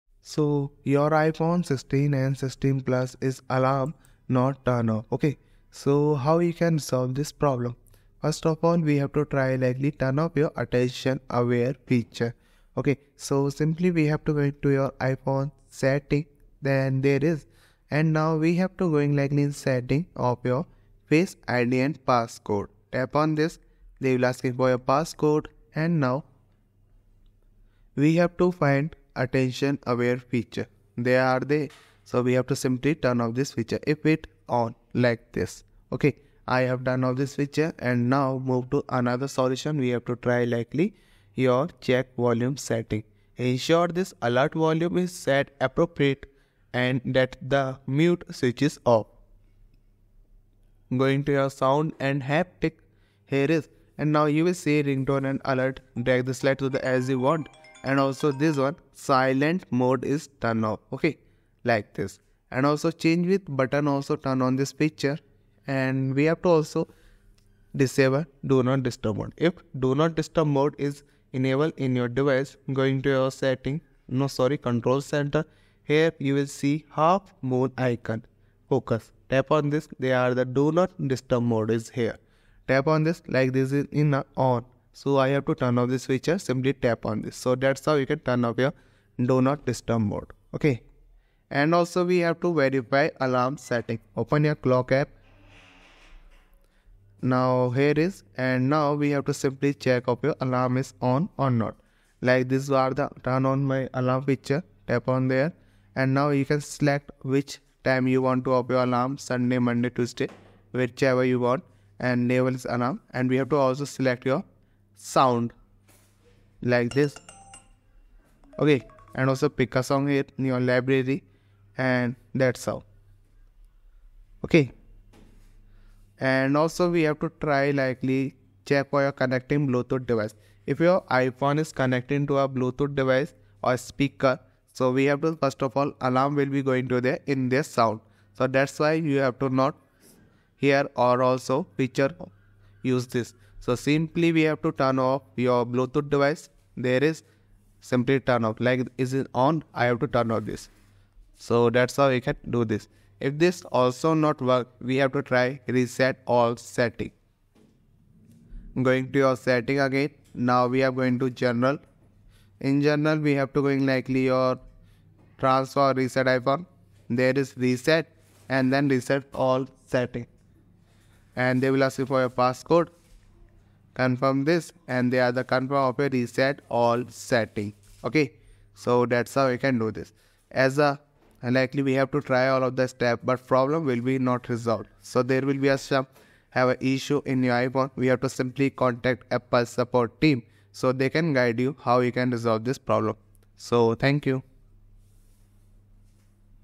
iPhone 16/16 Plus Alarm Not Going Off: Here’s How To Fix
In this informative video, we address a common issue faced by iPhone 16 and 16 Plus users: the alarm not going off. We will guide you through a series of troubleshooting steps to ensure your alarms function correctly.

00:10 Fix 1: Turn Off Attention Aware Feature
00:41 Fix 2: Check Volume Setting
01:08 Fix 3: Disable DND
01:31 Fix 4: Verify Alarm Setting
02:35 Fix 5: Reset